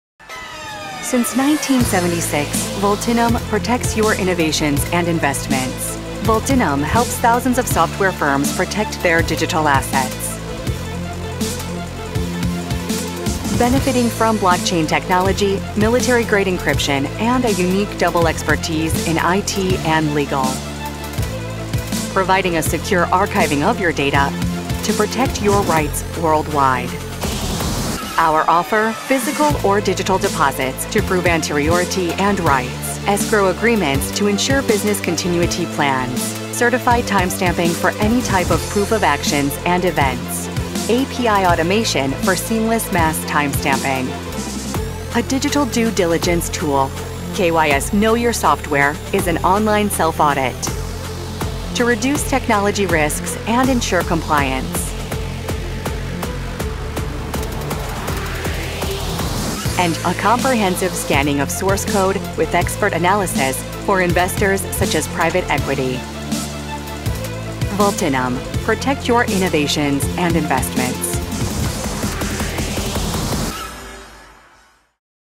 Vaultinum - Who are we?
Vaultinum, formerly known as Celog, is a trusted independent third party with years of experience in the protection of digital assets.

Today, Vaultinum makes a leap into the future, with new secured solutions that reflect the company's ambition to protect your innovations and investments .